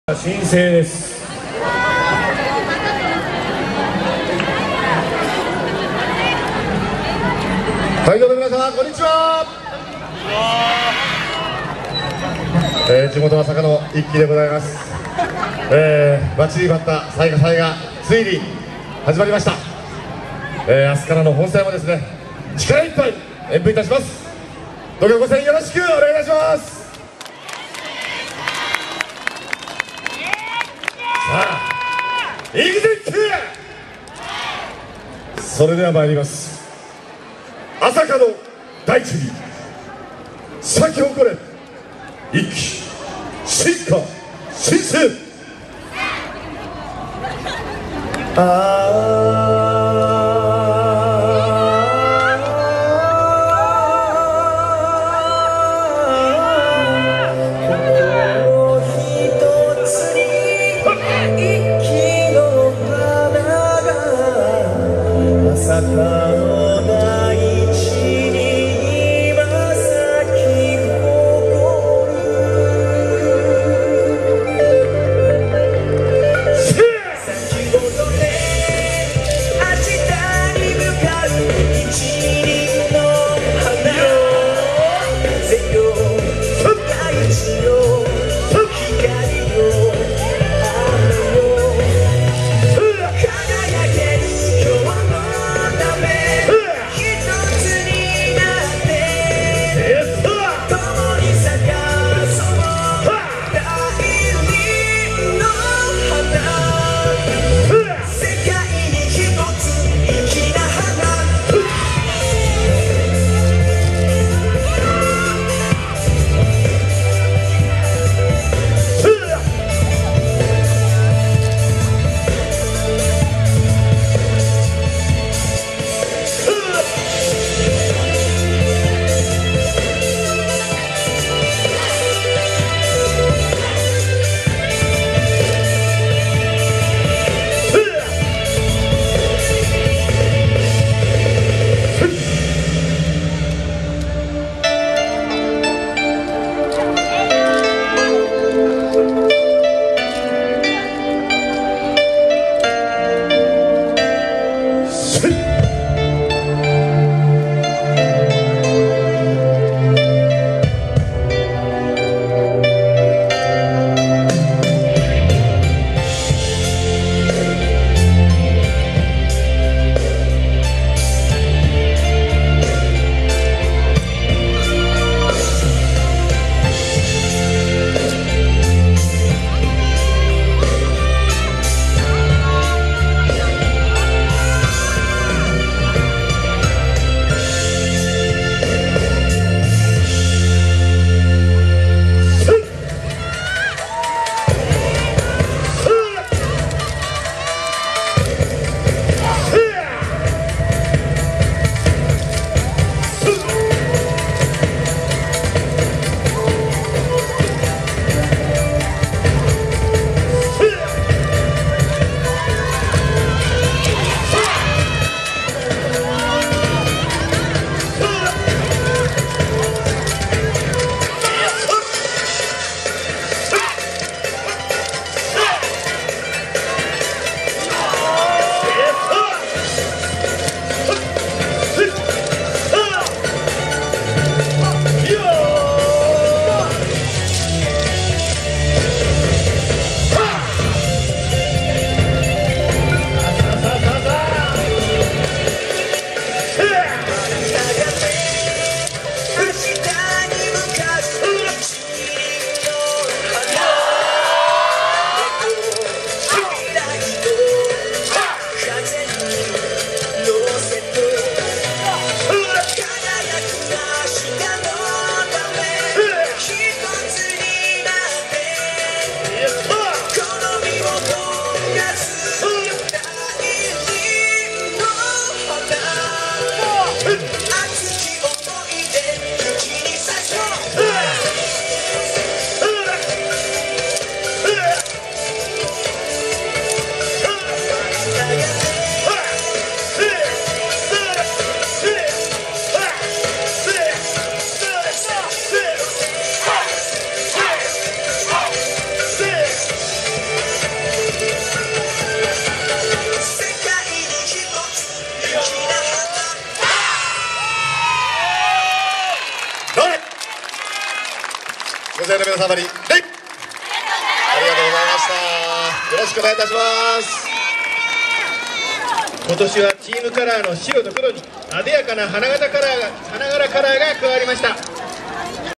粋（2014彩夏祭前夜祭・朝霞駅南口）
2014年8月1日 (金)～8月3日（日） に朝霞市で開かれた第31回朝霞市民祭り彩夏祭の前夜祭8月1日（金）に朝霞駅南口会場で演舞する「粋」（埼玉県 朝霞市）です。